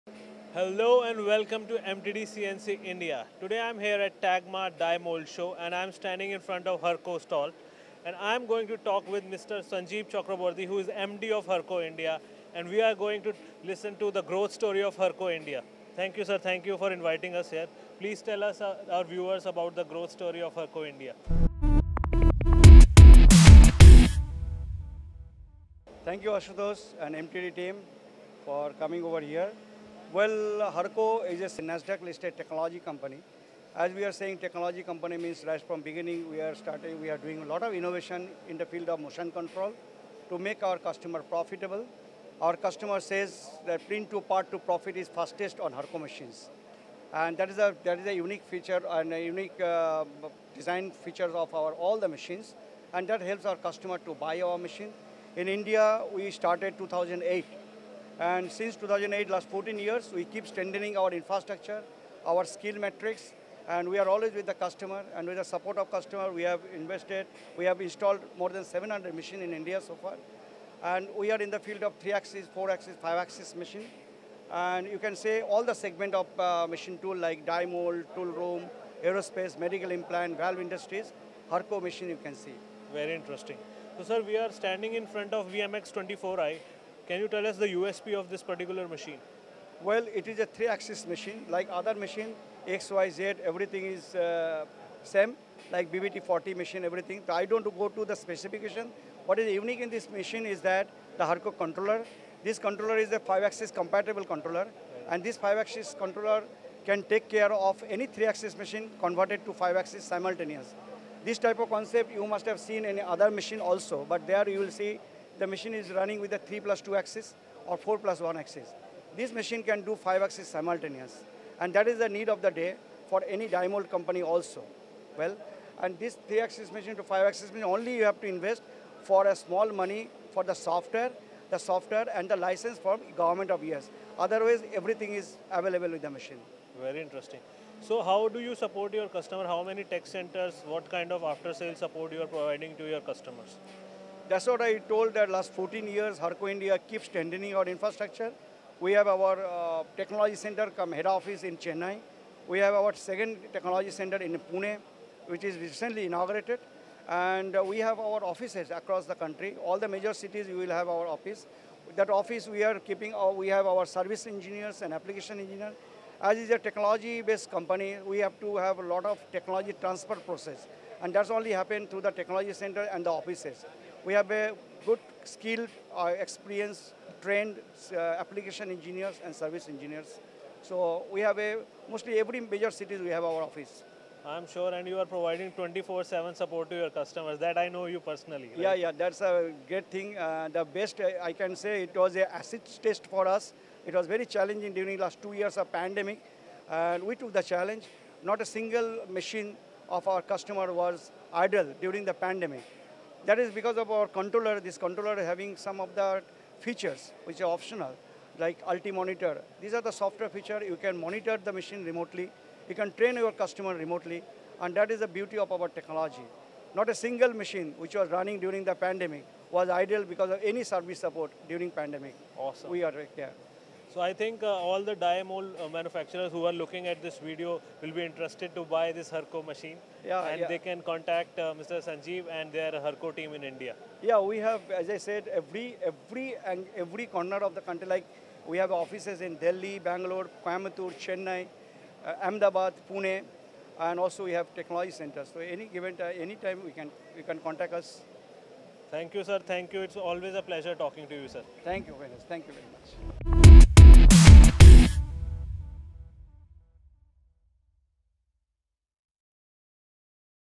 Lets learn about Hurco India
You will have seen across our channels content from Hurco in the UK and Europe but how does the global brand operate in other territories and in this instance, specifically India.